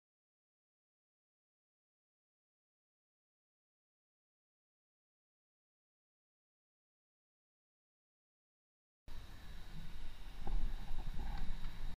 GV06 JNE Red light jump
The first car is ok, but it's the second car; it really jumps the red light.